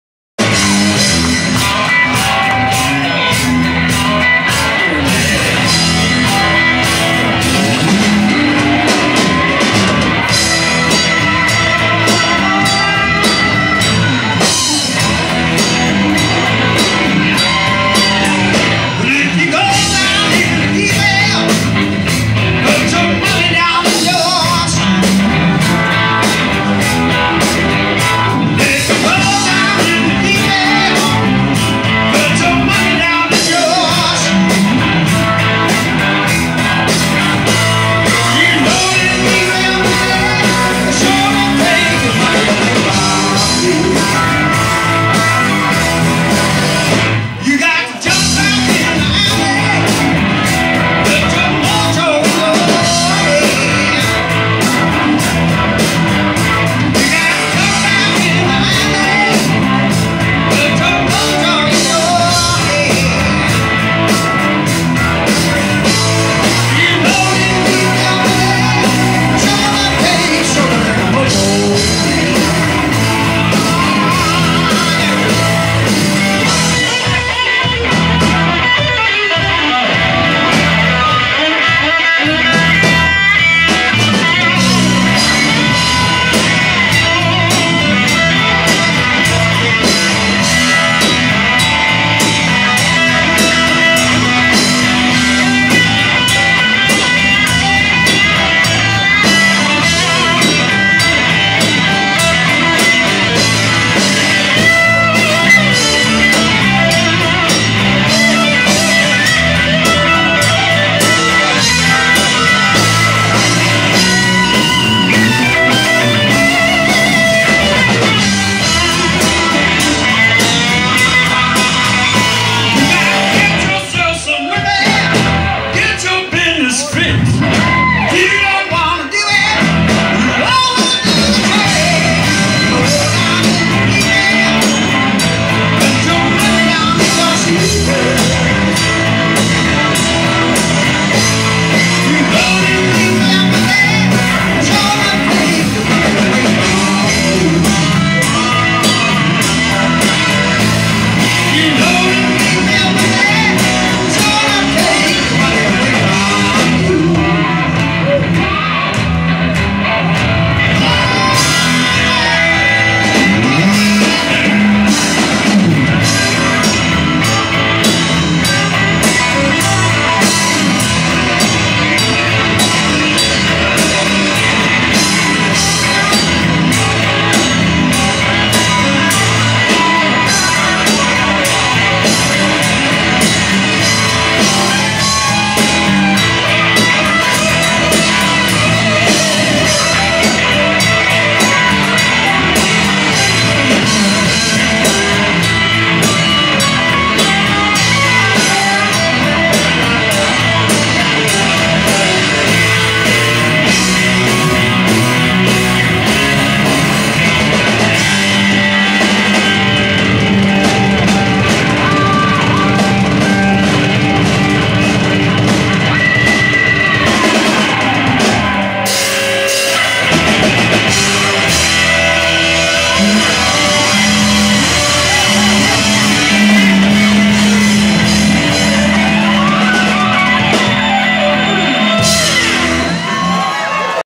Point Blank 2-27-11.wmv
Point Blank performs at Keys Lounge on 2/27/2011.